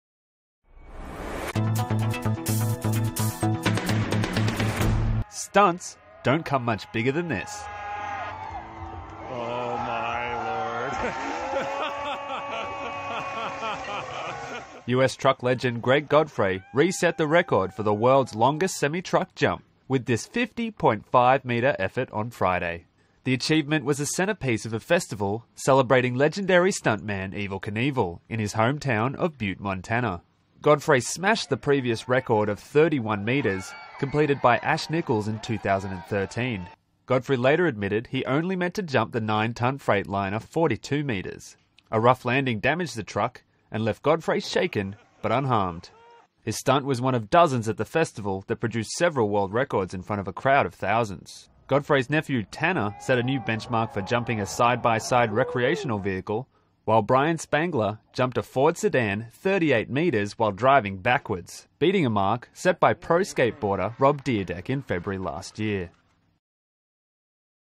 World record truck jump
Stuntman Gregg Godfrey jumps into the record books at Evel Knievel festival.